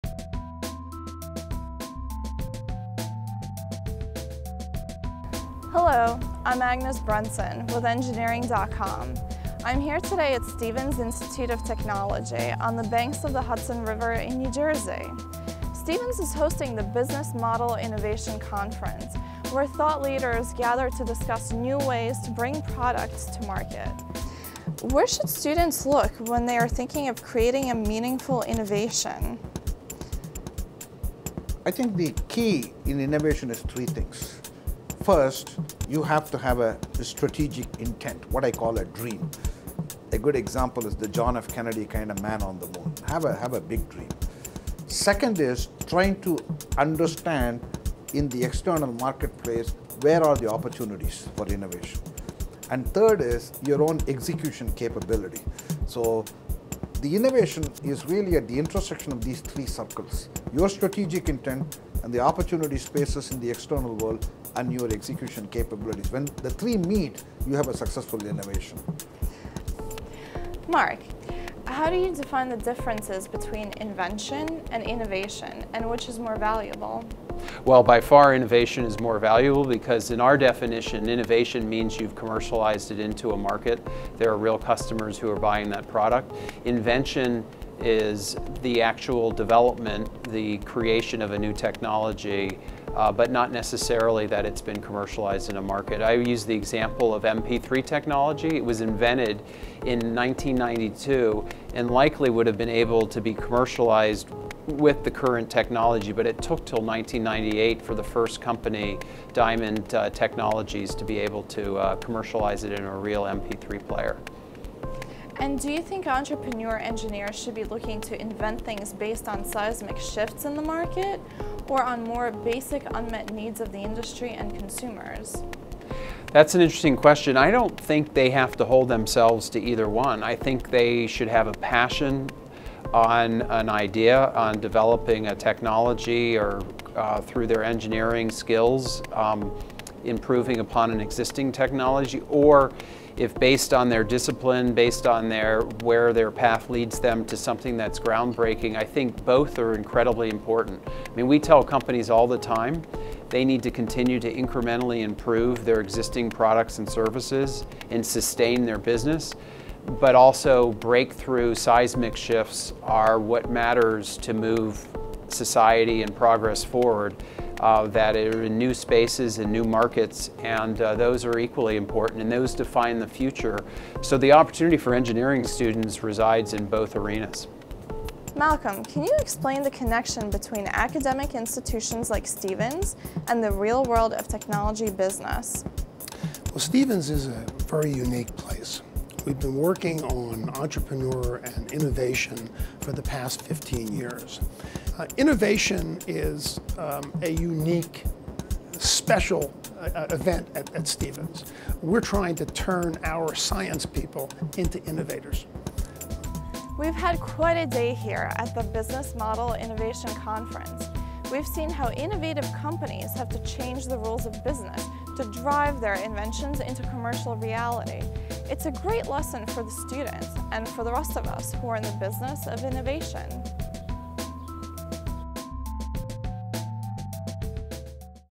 Stevens Institute of Technology: 2011 Business Model Innovation Conference
Join Stevens alum Agnes Brenson from Engineering.com as she speaks with the industry leaders from the recent Business Model Innovation Conference on the Stevens campus.  For more information please visit our sites: